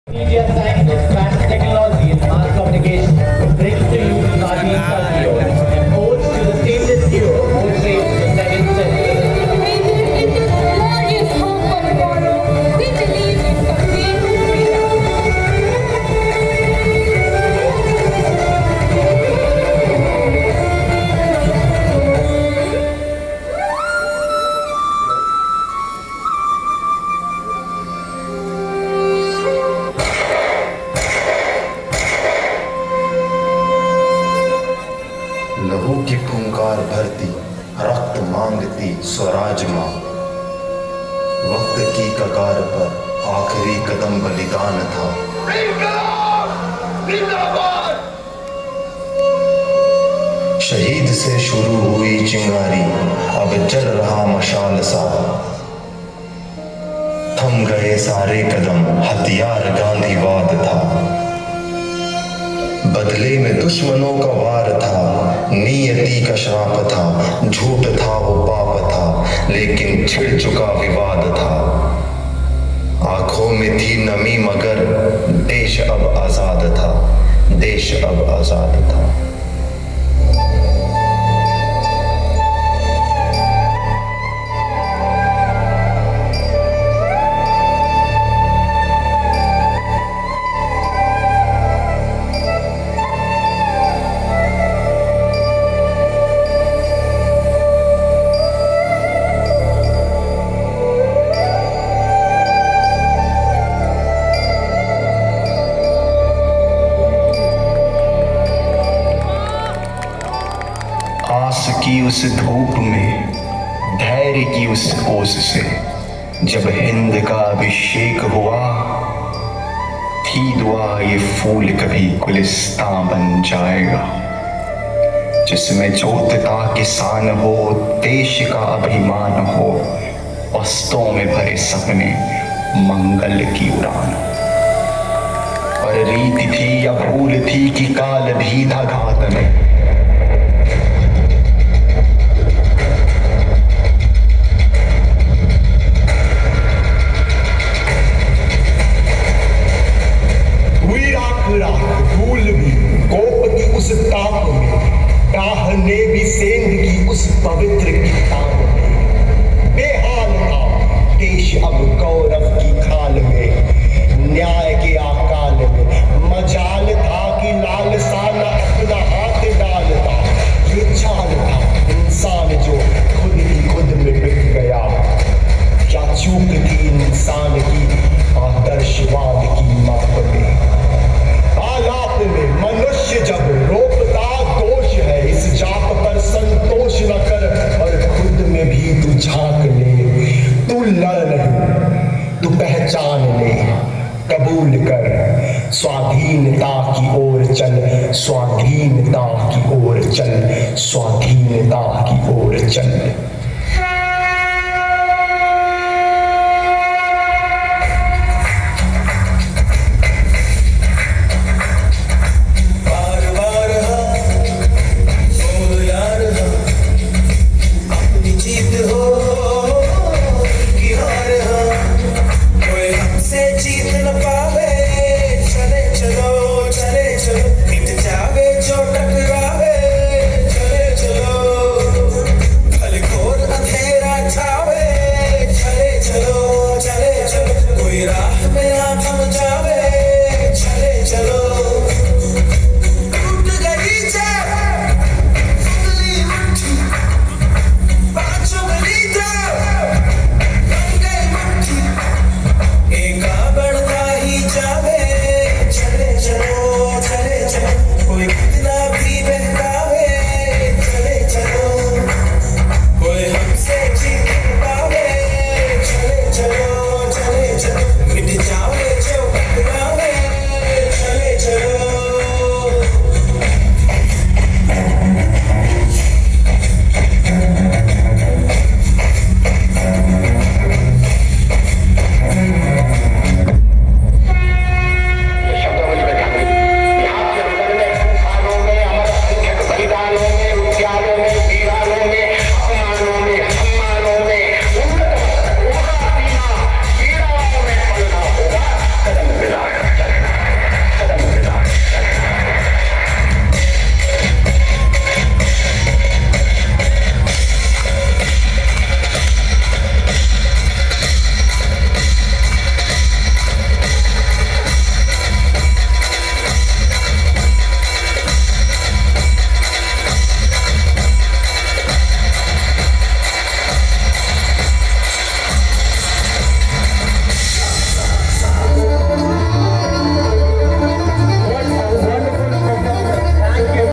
SFM, SFT & SMC performance during Republic Day celebrations in KIIT Deemed to be University
The performance got the 1st Prize in the show segment of 70th Republic Day programme.